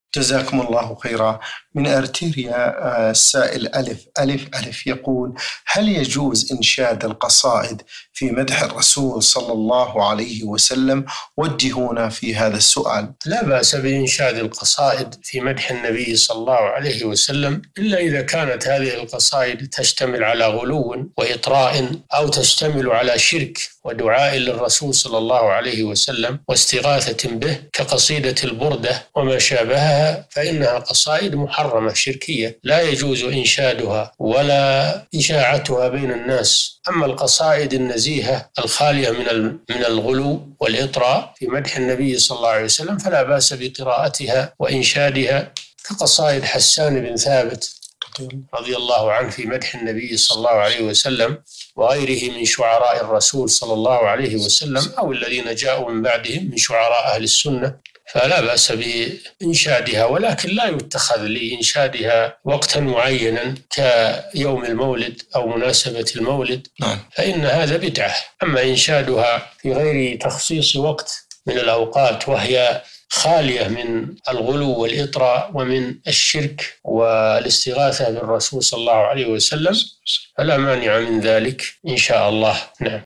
حكم إنشاد القصائد في مدح النبي صلى الله عليه وسلم - الشيخ صالح الفوزان
نص السؤال: هل يجوز إنشاد القصائد في مدح الرسول صلى الله عليه وسلم ؟ وجهونا في هذا السؤال.
السيرة الذاتية :
للاشتراك في قناة الشيخ صالح الفوزان

رئيس الفريق
لمشروع كبار العلماء